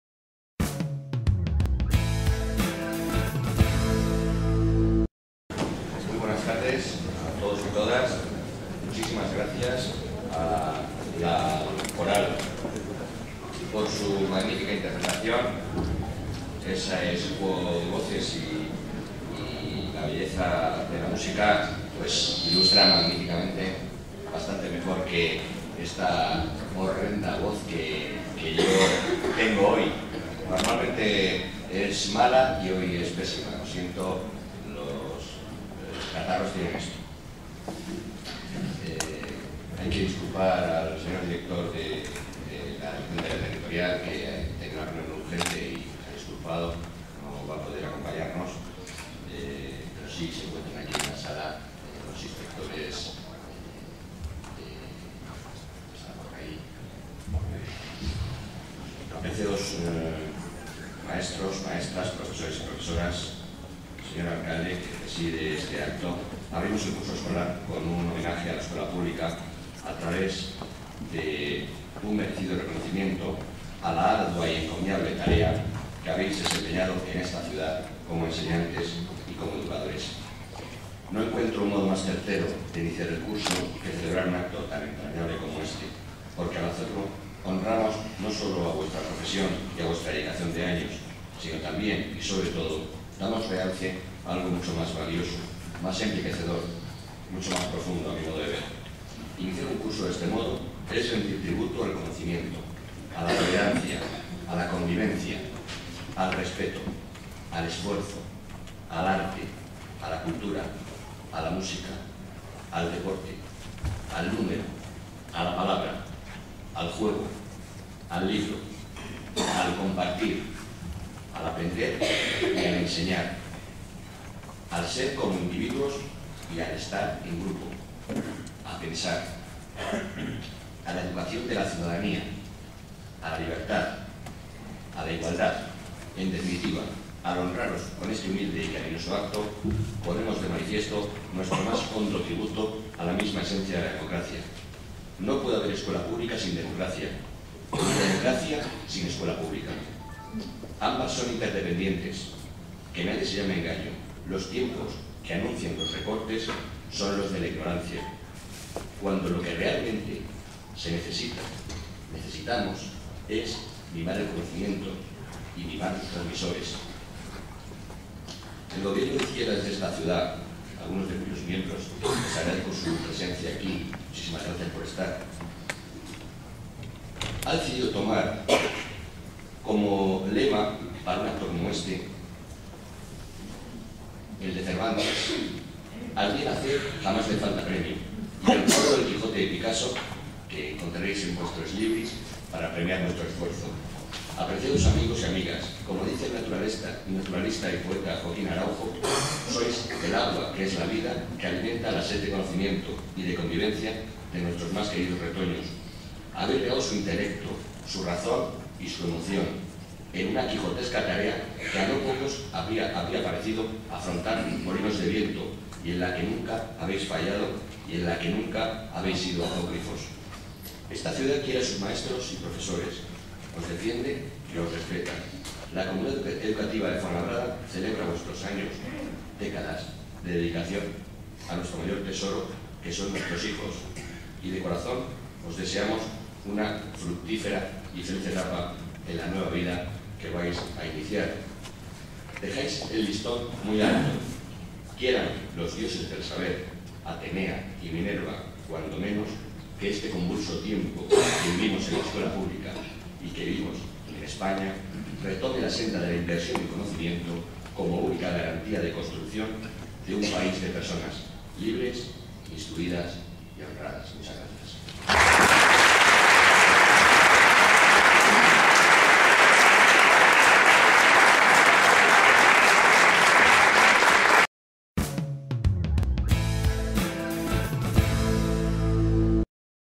Homenaje a la Escuela Pública. Intervención del Concejal de Educación
Homenaje a la Escuela Pública y a los profesores jubilados. Intervención de Isidoro Ortega, Concejal de Educación del Ayuntamiento de Fuenlabrada.